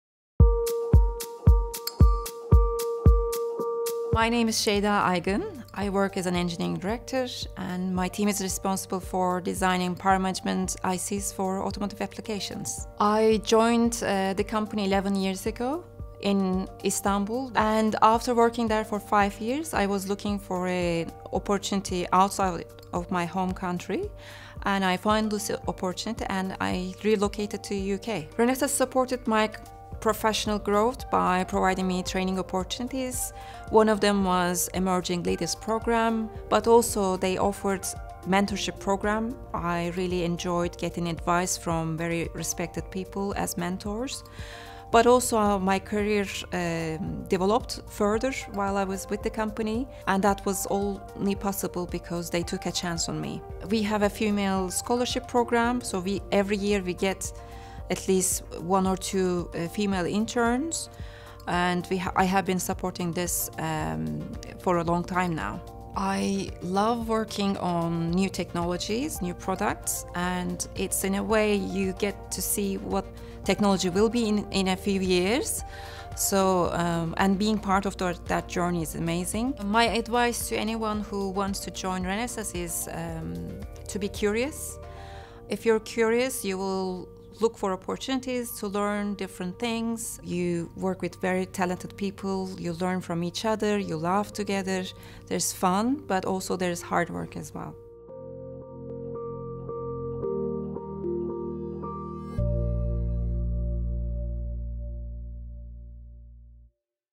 Seyda, Engineering Director, United Kingdom
As an Engineering Director, Seyda has moved with Renesas from Turkey to the UK. The support to move and grow as a leader has also included mentoring and leadership training. Now Seyda is enjoying the opportunity to support more women and interns coming into the industry.